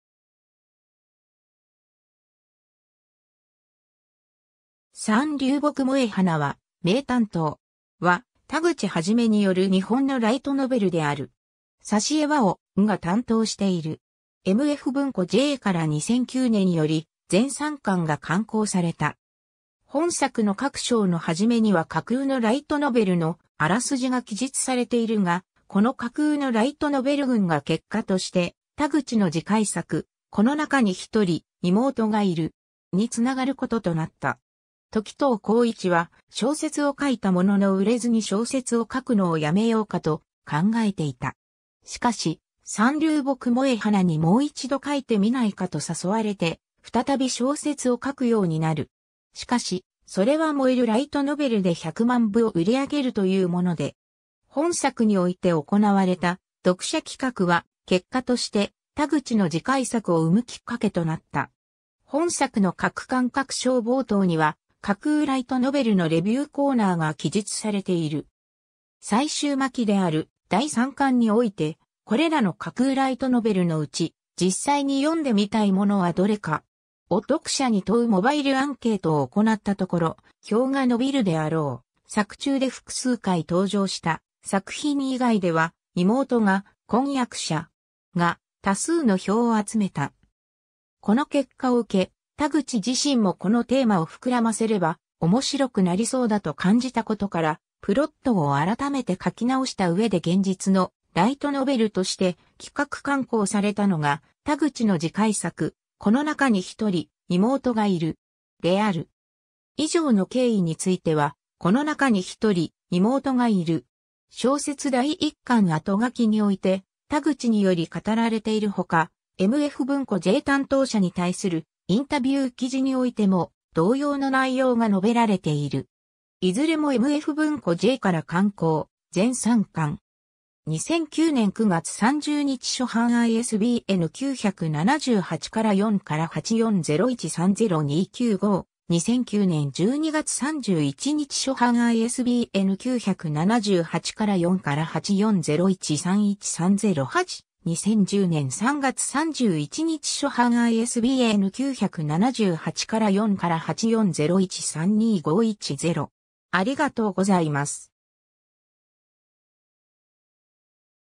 三流木萌花は名担当!
『三流木萌花は名担当!』（みつるぎもえかはめいたんとう）は、田口一による日本のライトノベルである。
挿絵はをんが担当している。
MF文庫Jから2009年より全3巻が刊行された。
 本作の各章の始めには架空のライトノベルのあらすじが記述されているが、この架空のライトノベル群が結果として田口の次回作『この中に1人、妹がいる!』につながることとなった。
 時任孝一は、小説を書いたものの売れずに小説を書くのをやめようかと考えていた。
しかし、三流木萌花にもう一度書いてみないかと誘われて再び小説を書くようになる。
しかし、それは萌えるライトノベルで100万部を売り上げるというもので・・・。
 本作において行われた読者企画は、結果として田口の次回作を生むきっかけとなった。
 本作の各巻各章冒頭には、架空ライトノベルのレビューコーナーが記述されている。
最終巻である第3巻において「これらの架空ライトノベルのうち、実際に読んでみたい物はどれか？」を読者に問うモバイルアンケートを行ったところ、票が伸びるであろう作中で複数回登場した作品以外では『妹が婚約者!?』が多数の票を集めた。
この結果を受け、田口自身もこのテーマを膨らませれば面白くなりそうだと感じたことから、プロットを改めて書き直した上で現実のライトノベルとして企画刊行されたのが、田口の次回作『この中に1人、妹がいる!』である。
 以上の経緯については、『この中に1人、妹がいる!』小説第1巻あとがきにおいて田口により語られているほか、MF文庫J担当者に対するインタビュー記事においても同様の内容が述べられている。
 いずれもMF文庫Jから刊行、全3巻。
 2009年9月30日初版 ISBN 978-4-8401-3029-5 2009年12月31日初版 ISBN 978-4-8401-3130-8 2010年3月31日初版 ISBN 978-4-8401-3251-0